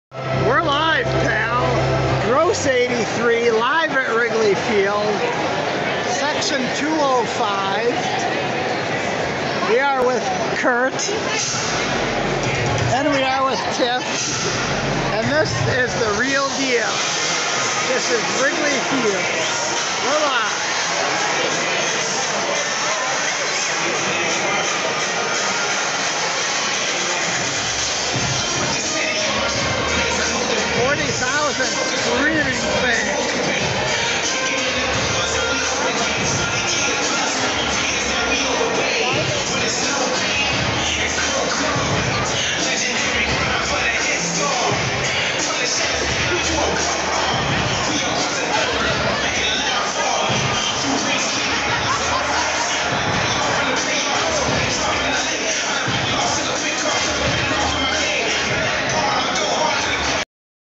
Cubs vs red Sox Saturday July 2nd 2022
A look at beautiful Wrigley field